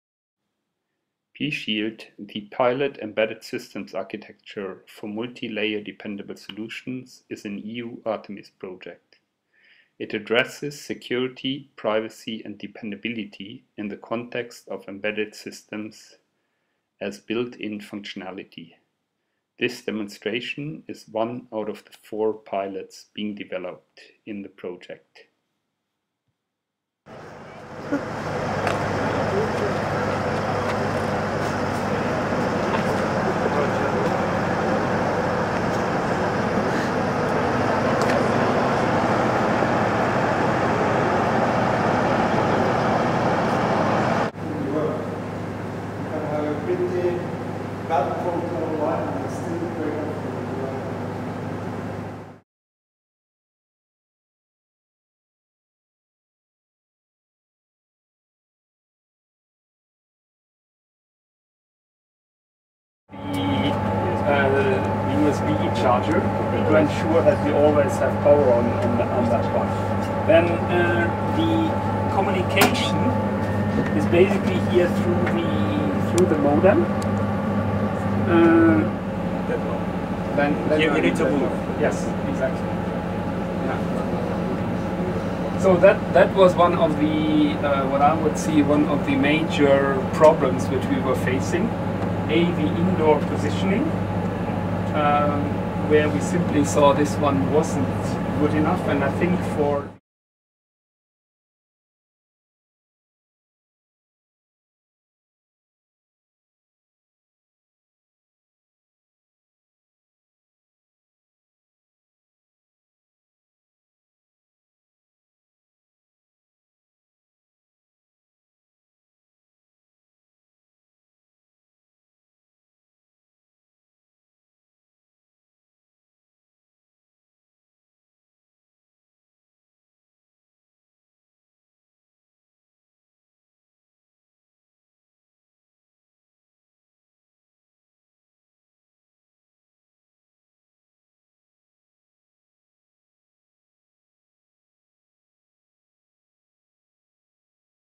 Artemis pSHIELD project - security for embedded systems on the JBV locomotive Roger
Imagine a world where 30-50 times more sensors than mobile phones are on the network, and where they all will provide data. Questions of security, privacy and dependability (SPD) will arise. Are the data really coming from my sensors? Did someone replace the sensor, og have hacked the data communication and inserted other values? Or just changed the sensor data by modifying the position. And if these sensors describe your private life, or even your health situation, would you trust a system which does not have security inbuilt?

This is what the pSHIELD project is all about. The project was the best ranked security project in the EU Artemis call on embedded systems, and aims at delivering "measurable SPD characteristics". However, such measures are not straight forward. The project needs to (i) identify a suitable description of the SPD parameters, (ii)  describe the envisaged security threats to the solution, (iii) identify counter measures and (iv) put the SPD features into a system.

This goal of pSHIELD is hard to achieve, thus the pilot SHIELD  (=pSHIELD) project concentrated on the prototypical implementation of key components of such a system:
* the decision maker on the algorithms being used
* hardware developments of radio interfaces, sensor and computer boards with are resistive to attacks, and
* the integration into a heterogeneous environment, consisting of a chain of sensors, control systems and applications being able to work with the data.

This video shows the implemented sensors on the JBV (National Rail Administration of Norway) measurement locomotive "Roger", visited by the project team in October 2011. The data from the embedded systems are transferred into the Telenor Objects M2M (machine-to-machine) platform "Shepherd". From Shepherd users can access the sensor data, and build their value-added services on top of the data. Our goal of demonstrating interoperability in the secure exchange of sensor data was reached. A mathematical decryption of security information can be achieved, but security handling is still a topic of trust and transitive trust. Thus the follow-on project nSHIELD will look at algorithms being used to increase privacy of data, and enhance the capabilities of the included boards.